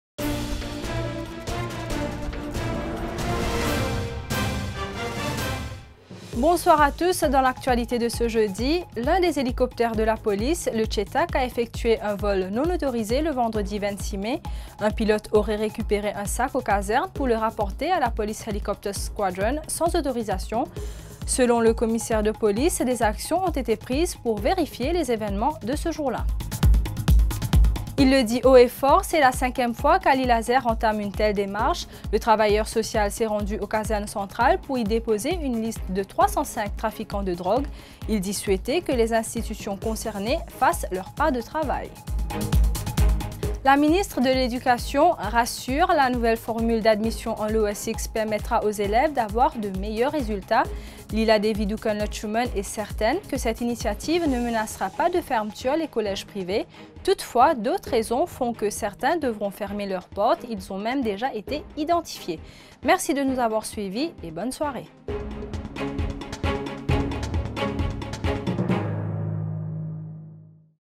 LaMinuteInfo: vol mystérieux d’un hélicoptère de la police
A la une de LaMinuteInfo de ce jeudi 1er juin, l’hélicoptère «Chetak» aurait effectué un vol non autorisé le vendredi 26 mai. Egalement dans l’actualité, Ally Lazer dépose une liste de marchands de drogue.